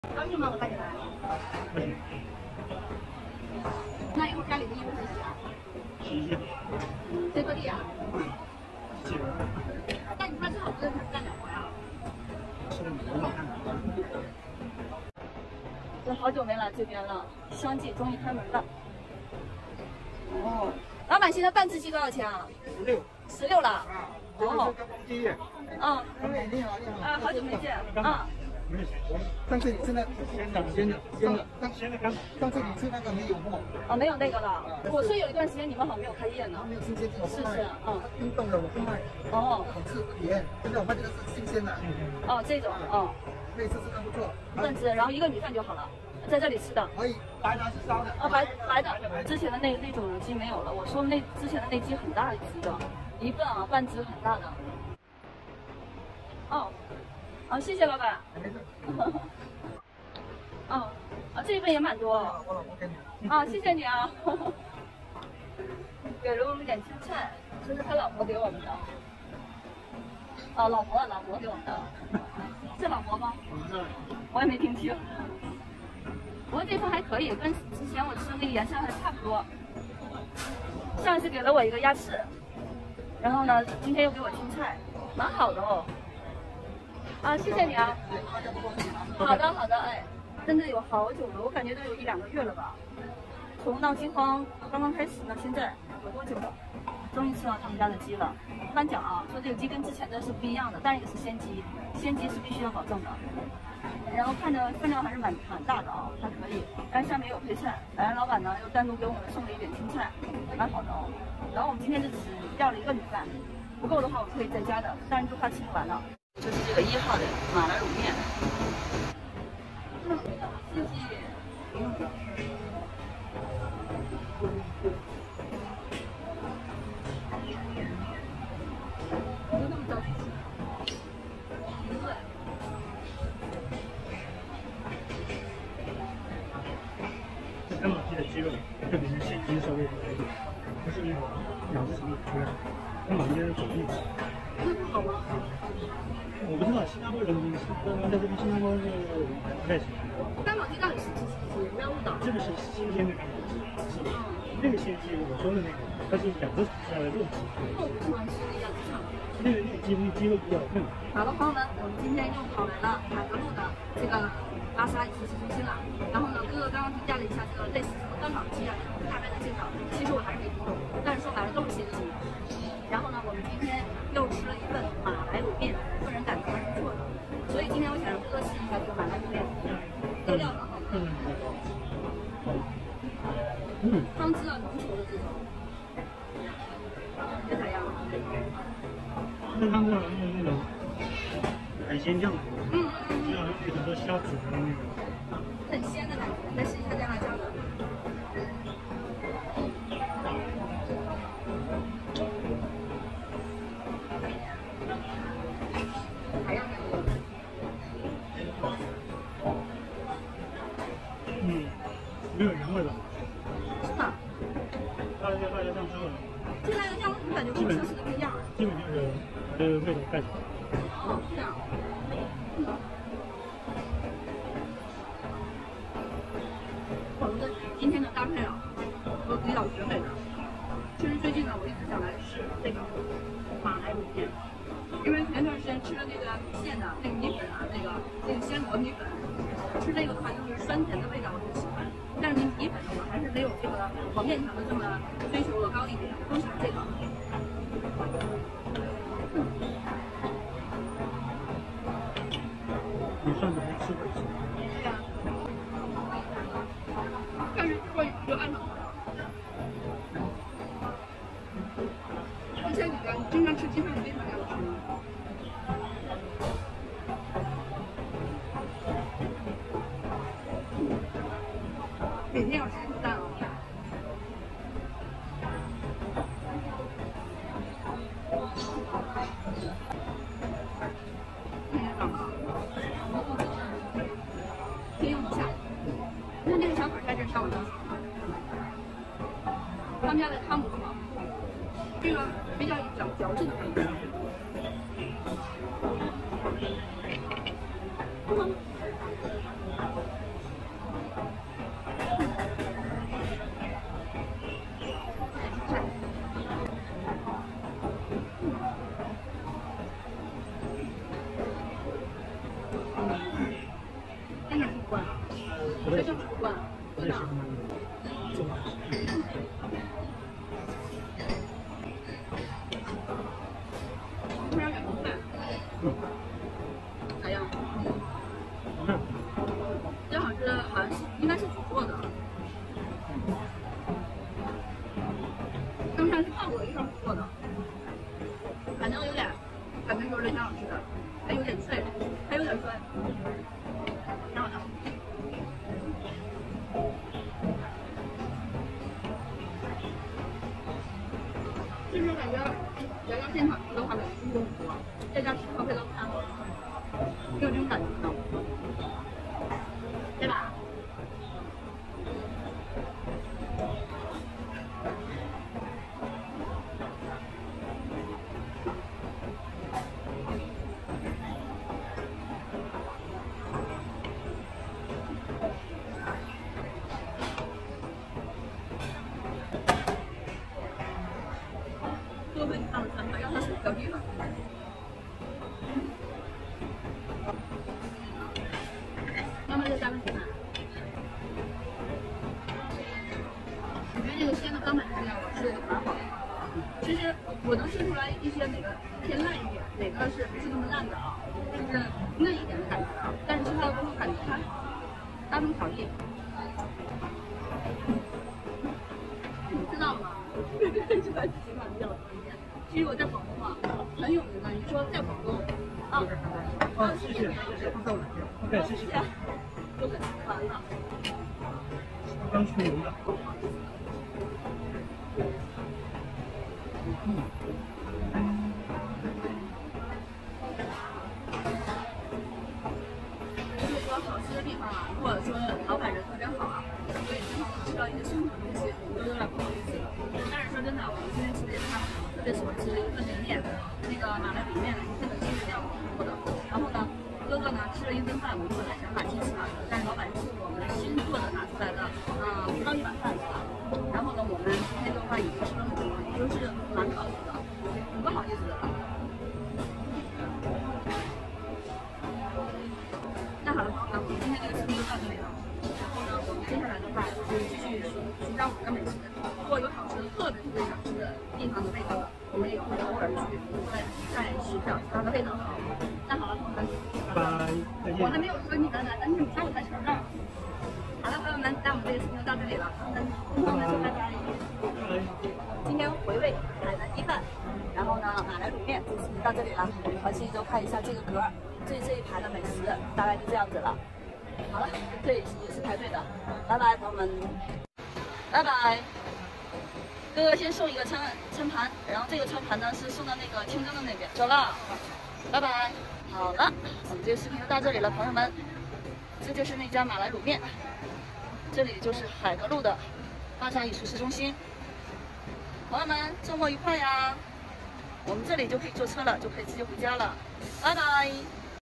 新加坡芽笼士乃 海格路熟食中心 Geylang Serai, Singapore. Chicken rice, Mee rebus 带老公吃甘榜海南鸡饭“鸡不如之前” 必吃我喜爱的马来卤面
半只鸡两碗鸡饭哥哥吃的很香，马来卤面哥哥平价不错却不是他喜爱的美味，我吃到久违的味道好满足哦。